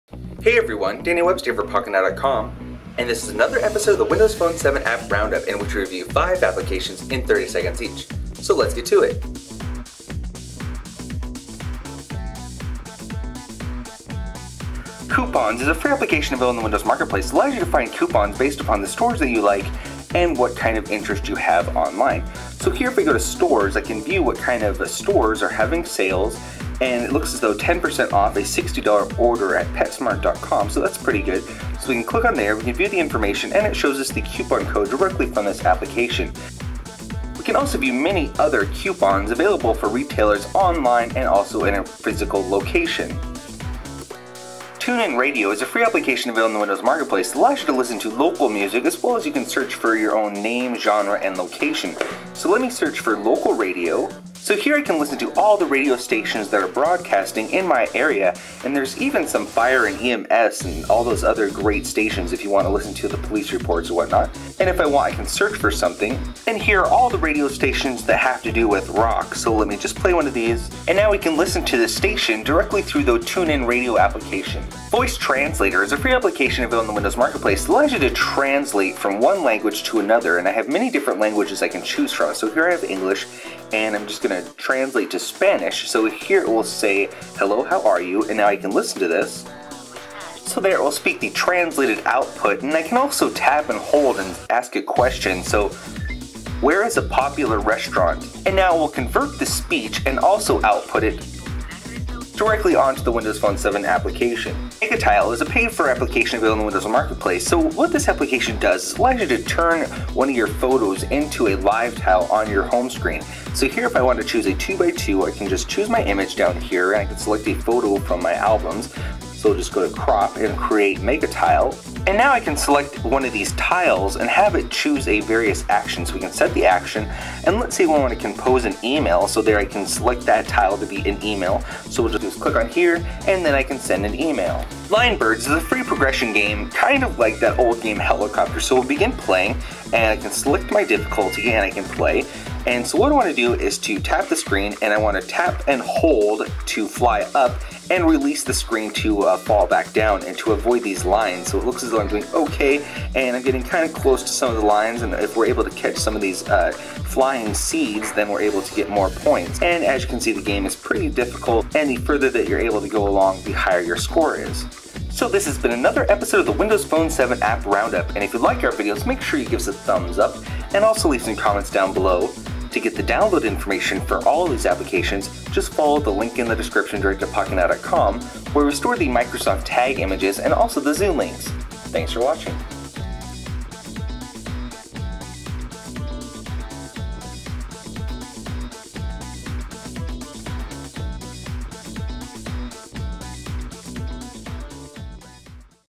Windows Phone 7 App Roundup - Coupons, Tunein Radio, VoiceTranslator, MegaTile, Linebirds
In this episode of the Windows Phone 7 App Roundup we demonstrate five applications in 30 seconds each. These apps will keep you entertained, help you translate using your voice, and get great deals with online coupons. To download these apps directly to your WP7 device, just click on the title of the application and the Zune software will automatically launch to the app in Marketplace. If you have the free Microsoft Tag app installed on your WP7 smartphone just snap a picture of the Tag image and the mobile Marketplace will navigate to that app. This week we have Coupons 0:16, Tunein Radio 0:46, VoiceTranslator 1:16, MegaTile 1:46, and LineBirds 2:16.